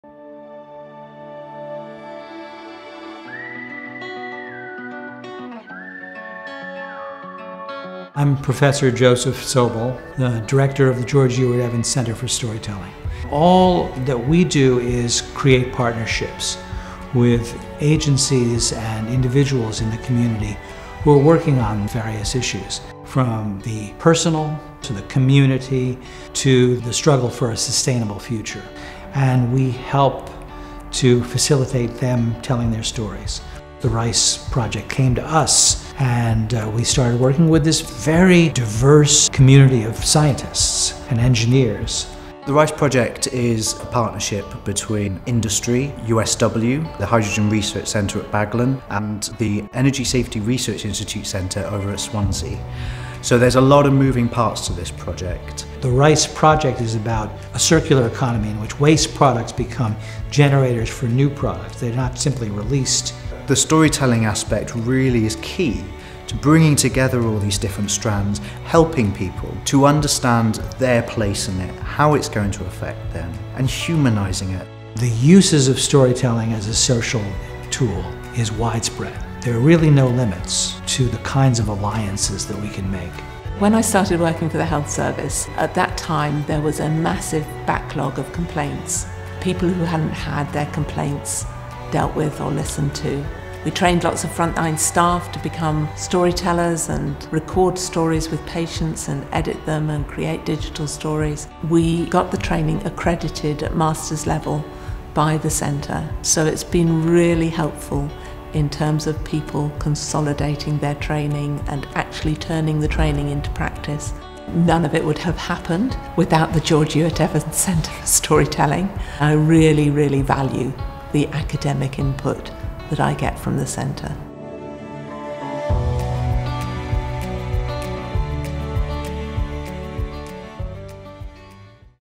USW Research Impact | Storytelling for Wellbeing
Environmental action, women’s wellbeing, and healthcare sector initiatives often fail to get their messaging across to potential stakeholders. Research and storytelling methodologies developed by the George Ewart Evans Centre for Storytelling (GEECS) at the University of South Wales (USW) have helped bridge communication gaps, promote understanding, and enhance environmental, community, and individual health and wellbeing.